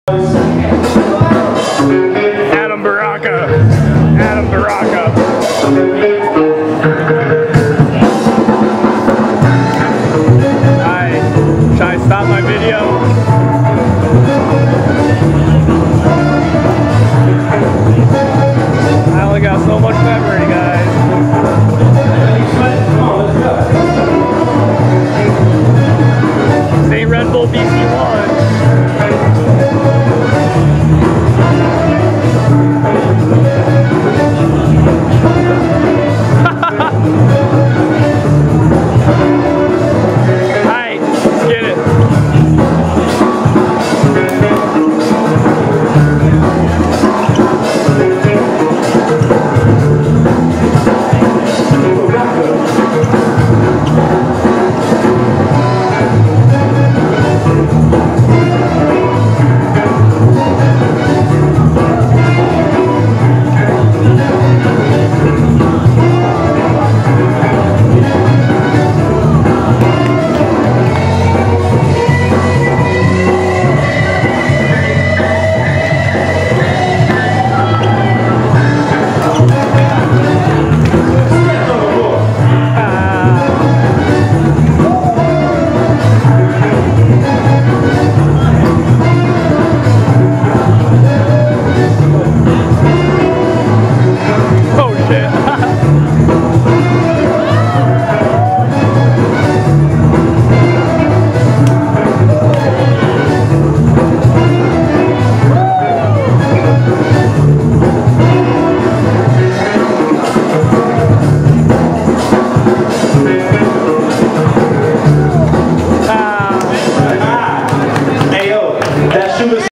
Baraka vs Sketch || Top 16 || Flint Town Get Down 2016
Vertical Ambition Dance Company & Creative Expressions Dance Studio Presents:

FLINT TOWN GET DOWN 2016!

BREAKING 1v1:
1st: $200 and Custom Plaques + Medallions (WELL DONE INC)
2nd: $50 and Custom Plaques

DJ: Spuratik (IMS - Detroit, MI)
MC: J-Aura (Vertical Ambition - Flint, MI)
Judges:
B-girl Shortee Roc (Vertical Ambition - Flint, MI)
B-boy Marvee (Robotops/ IMS - Detroit, MI)
B-boy Leachy (Hardcore Detroit - Detroit, MI)